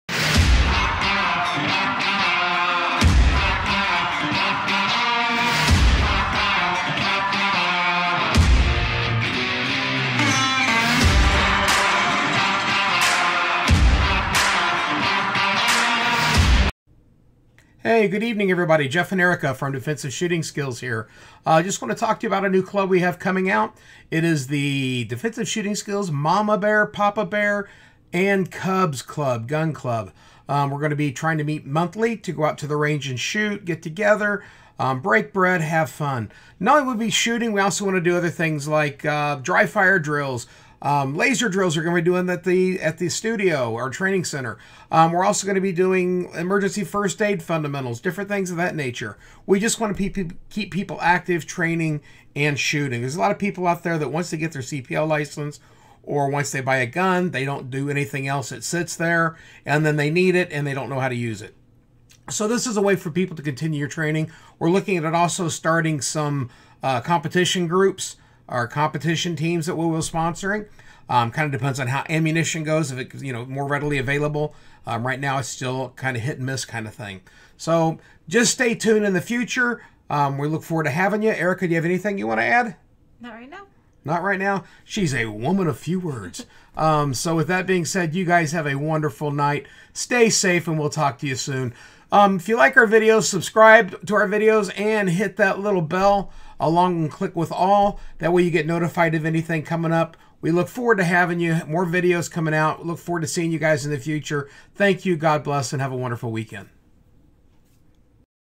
Defensive Shooting Skills Presents The Bears Shooting Club
Defensive Shooting Skills would like foy to follow us as we start a gun club.